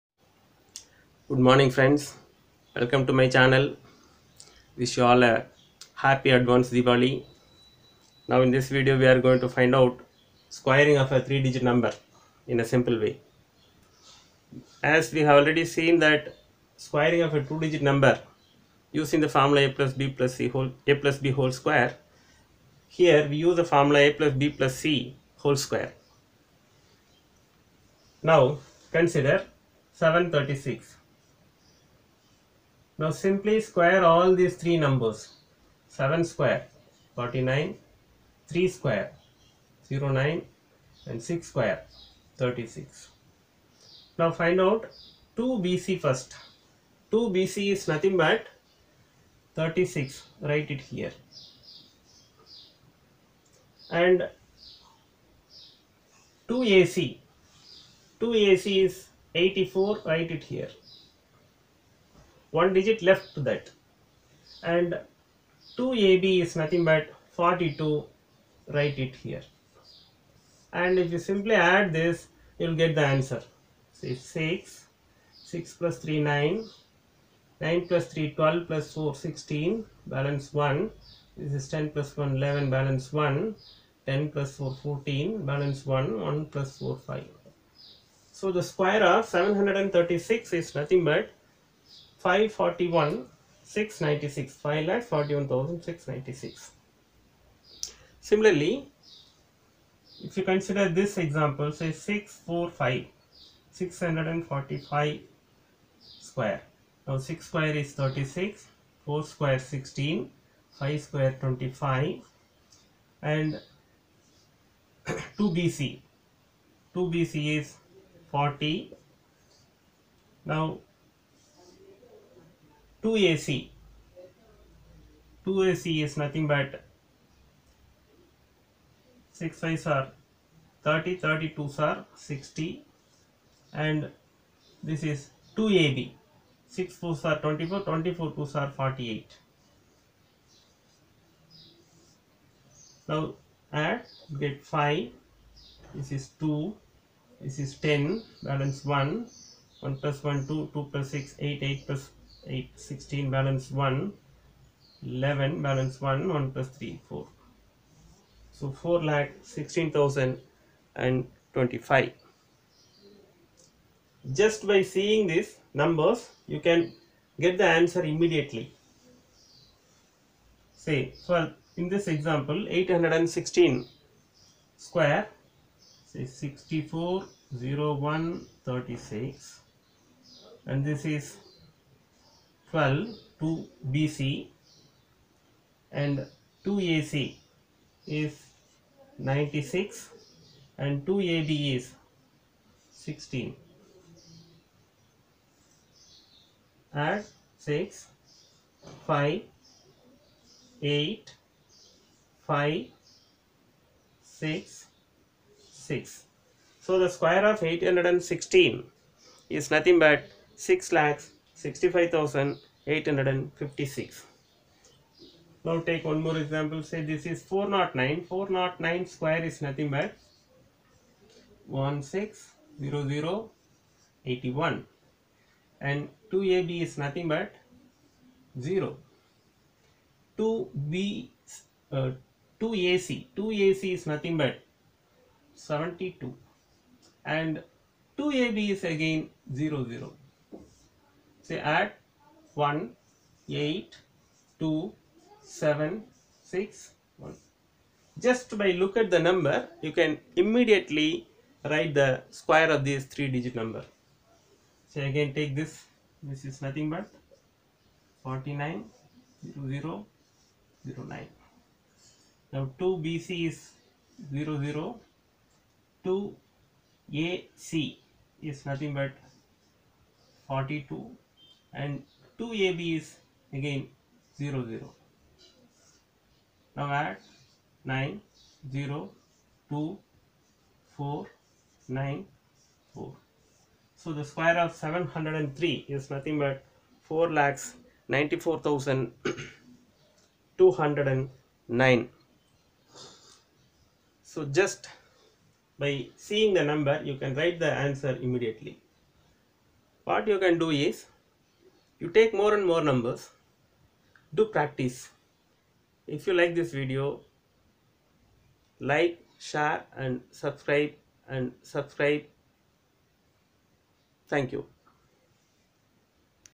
Squaring a 3 digit number in a few seconds using the formula ( a + b + c )^2.....
In this video we are going to see how to find square of a 3 digit number using the formula ( a + b + c )^2 with rapid speed...